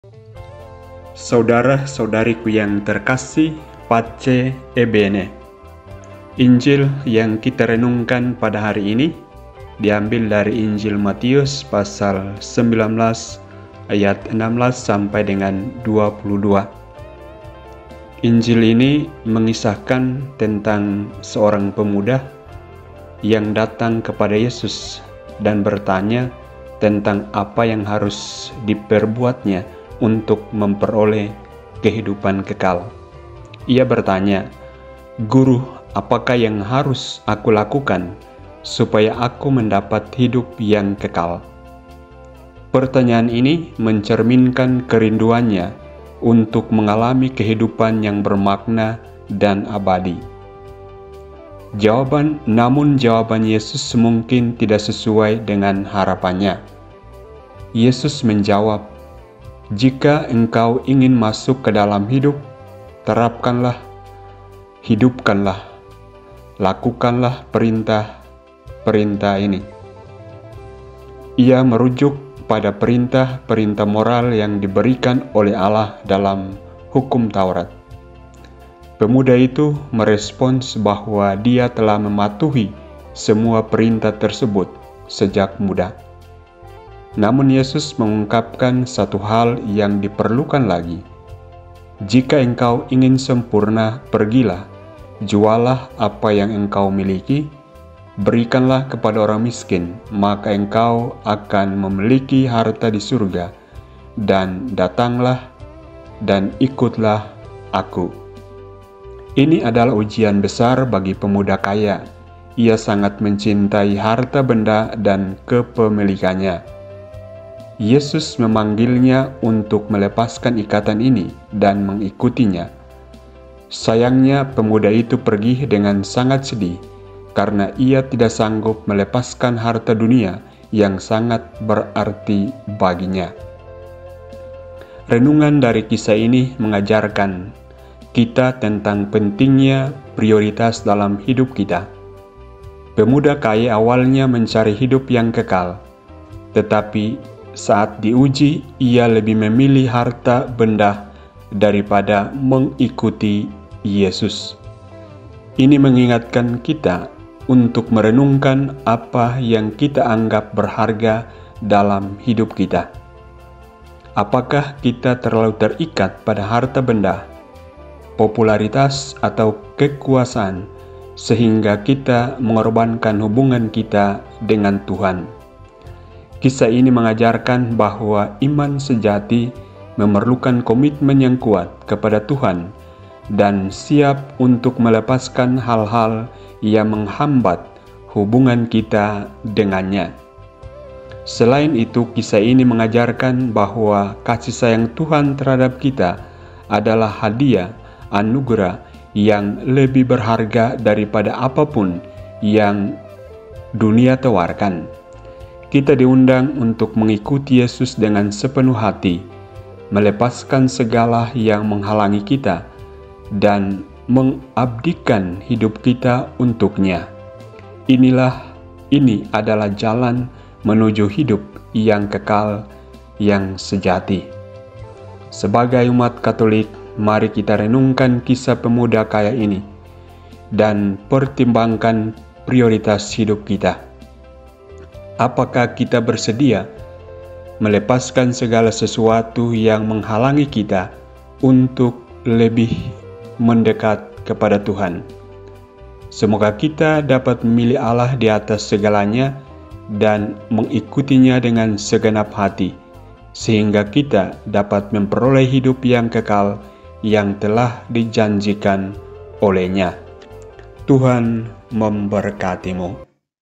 RENUNGAN HARIAN KATOLIK, SENIN 21 AGUSTUS 2023
Kelekatan pada harta duniawi membuat orang susah untuk mengikuti Yesus.